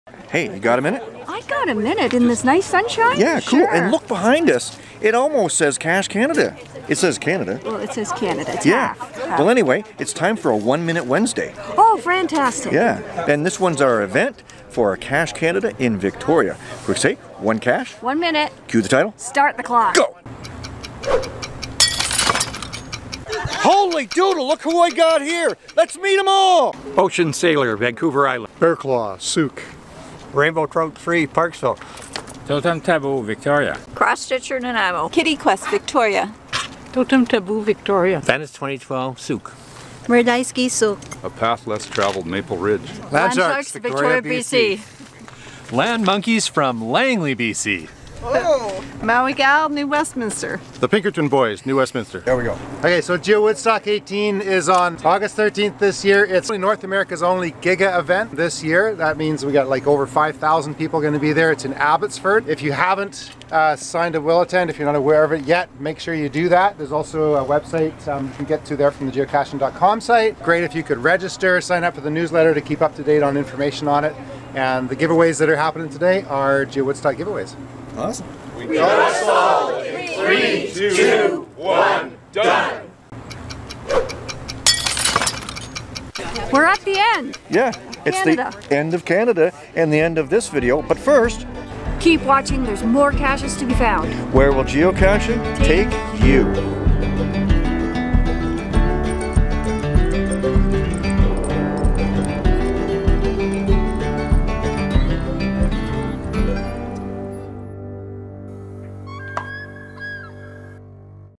One Victoria Minute
We held an event when we were in Victoria and met some great people. Nice to meet the locals. We also met the Landsharkz and the LANMonkeys!